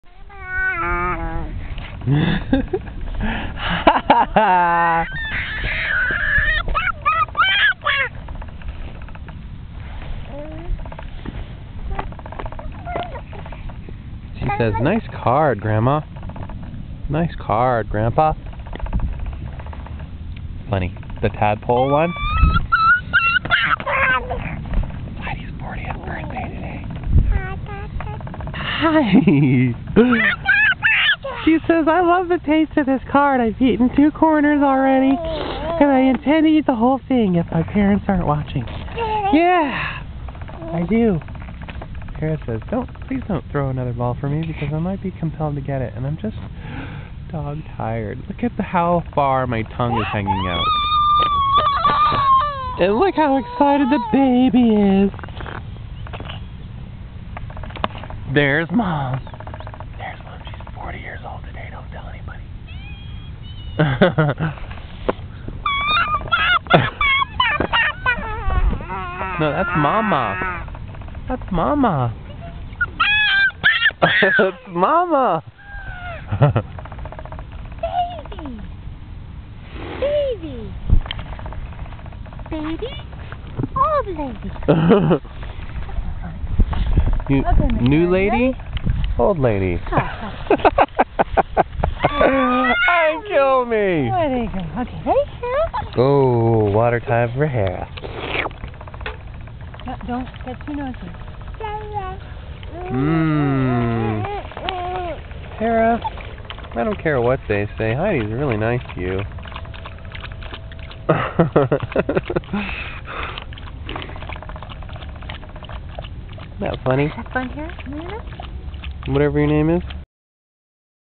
Abbott Family at Tilden
Robin, Heidi, Amanda, and Hera hang out at Tilden park in front of the Brazil Building on the afternoon of Heidi's 40th.